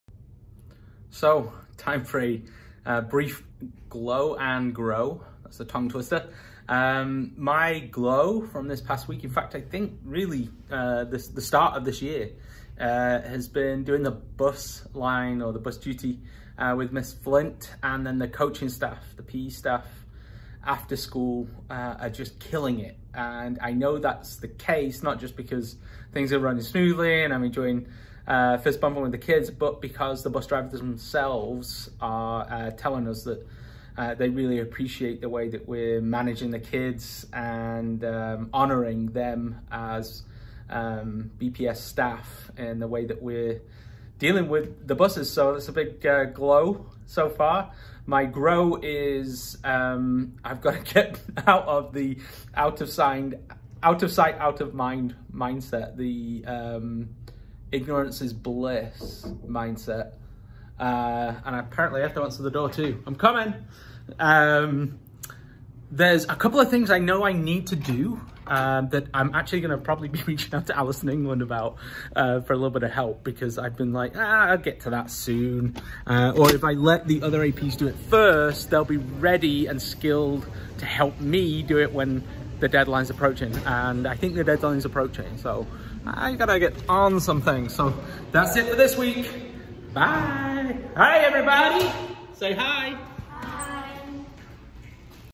Reflection | Week 6: Bus Lines and Deadlines
This week I reflect on the accolades that the bus drivers are giving to our staff and lament my "ignorance is bliss" mindset. Deadlines are coming and it's time to get in gear.